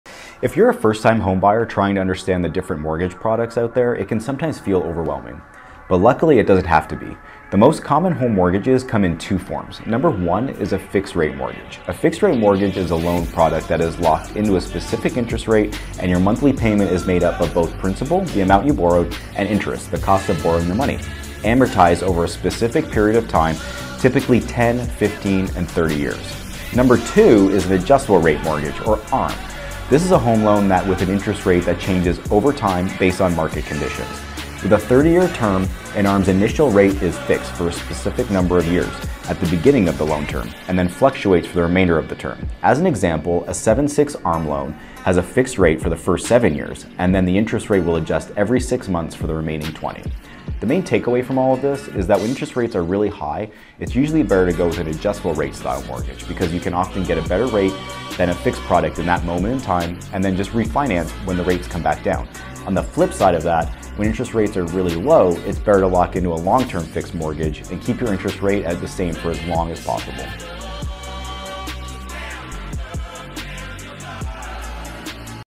2 Most Common Mortgage Products
If you are a first time home buyer, trying to understand the different mortgage products out there can sometimes feel overwhelming, but luckily it doesn’t have to be. The most common home mortgages come in two forms:
Fixed Rate Mortgage: A fixed rate mortgage is a loan product that is locked into a specific interest rate and your monthly payment is made up of both principle  (the amount borrowed) and Interest (the cost of borrowing the money) amortised over a specific period of time typically 10, 15 & 30 Years.
Adjustable Rate Mortgage or ARM and this is a home loan with an interest rate that changes over time based on market conditions. With a 30-year term, an ARM’s initial rate is fixed for a specified number of years at the beginning of the loan term and then fluctuates for the remainder of the term. As an example a 7/6 ARM loan has a fixed rate for the first 7 years, then the interest rate will adjust every 6 months for the remaining 20 years.

Takeaway: When Interest rates are high it is usually better to go with an adjustable rate style mortgage because you can often get a better rate than fixed rate products at that moment and refinance when rates come back down. On the flip side, when interest rates are really low it is better to lock into a long term fixed rate mortgage and keep your interest rate the same for as long as possible.